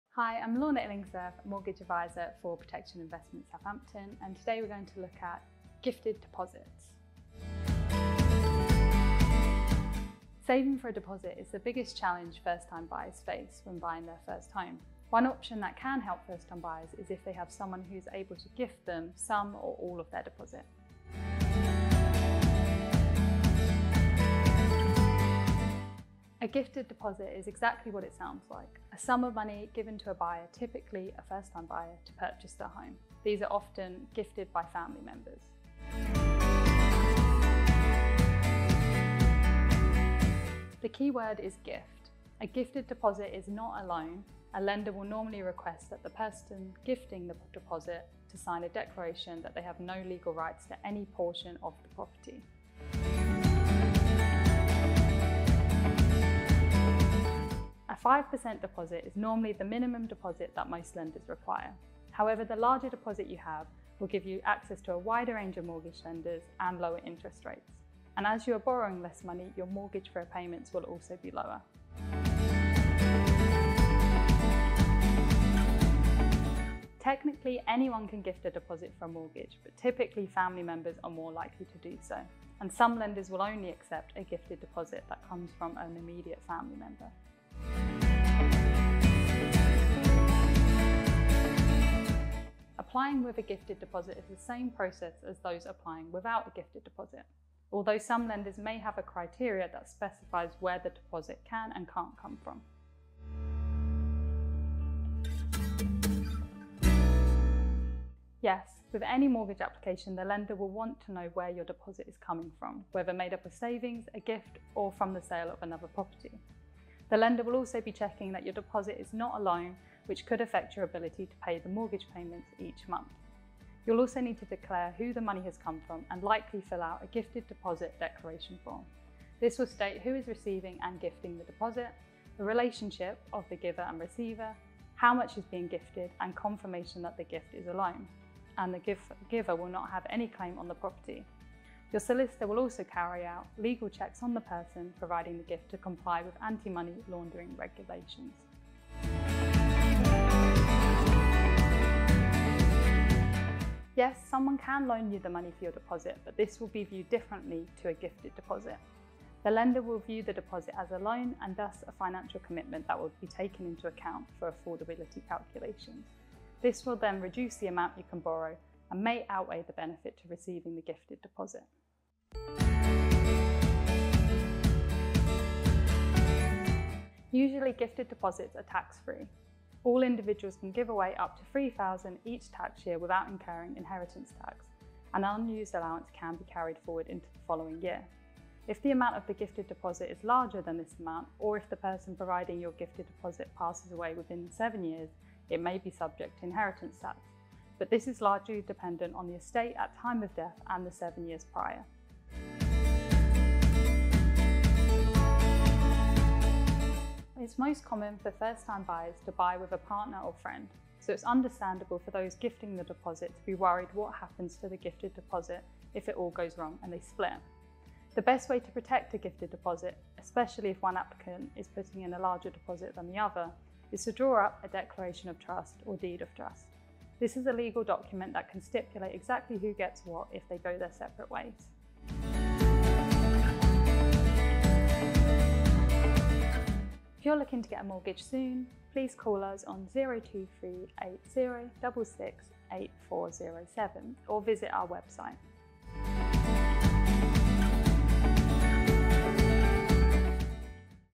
Gifted Deposits
Timestamps
0:00 Introduction
0:07 Gift Deposits
0:21 What is a Gifted Deposit
0:36 How do Gifted Deposits work?
0:53 Why is a bigger deposit better?
1:12 Who can gift a deposit?
1:28 How do I get a mortgage with a Gifted Deposit?
1:43 Do I have to declare a Gifted Deposit?
2:29 Can the deposit be loaned instead of gifted?
2:52 What are the tax implications of a Gifted Deposit?
3:22 How to protect a gifted deposit
3:52 Contact Protection & Investment